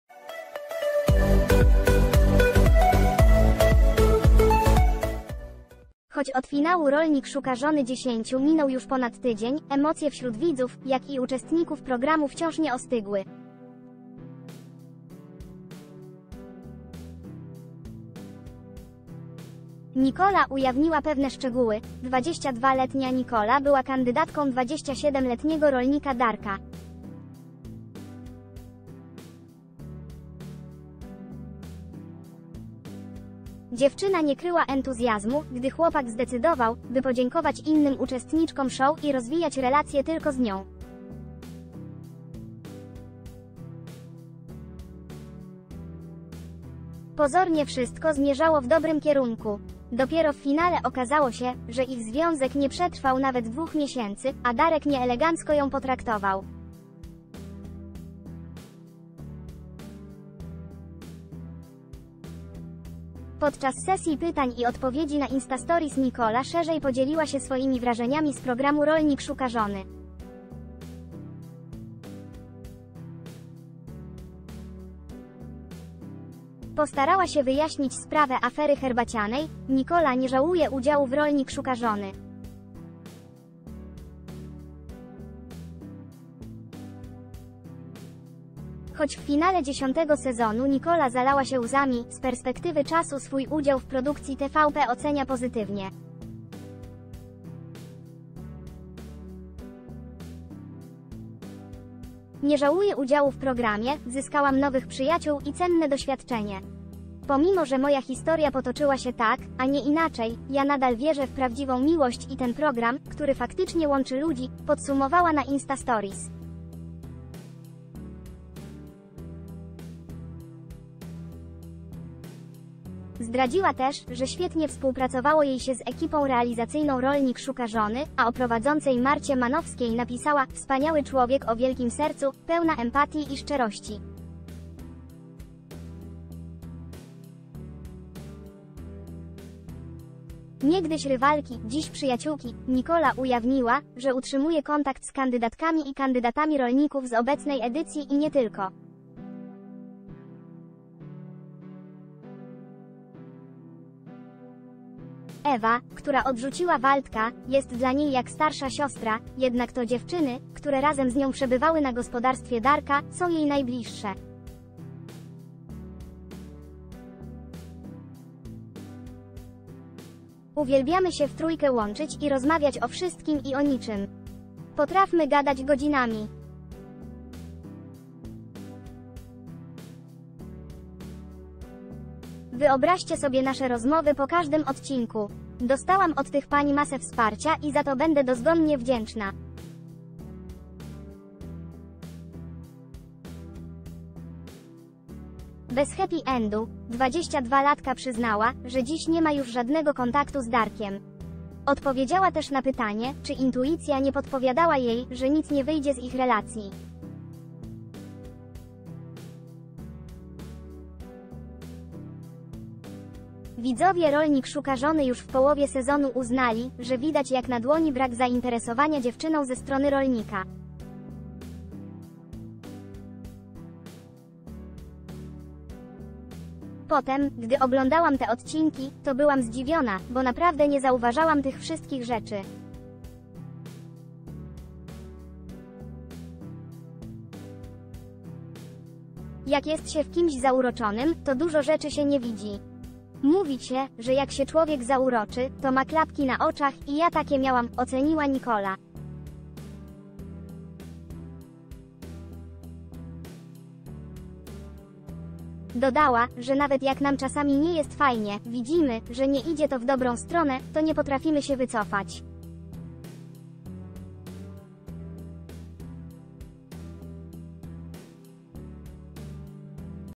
Dopiero, gdy zobaczyła w TVP jej relację z Darkiem, wszystko pojęła Miałam klapki na oczach
Choć od finału "Rolnik szuka żony 10" minął już ponad tydzień, emocje wśród widzów, jak i uczestników programu wciąż nie ostygły. Nicola ujawniła pewne szczegóły.
22-letnia Nicola była kandydatką 27-letniego rolnika Darka. Dziewczyna nie kryła entuzjazmu, gdy chłopak zdecydował, by podziękować innym uczestniczkom show i rozwijać relację tylko z nią. Pozornie wszystko zmierzało w dobrym kierunku. Dopiero w finale okazało się, że ich związek nie przetrwał nawet dwóch miesięcy, a Darek nieelegancko  ...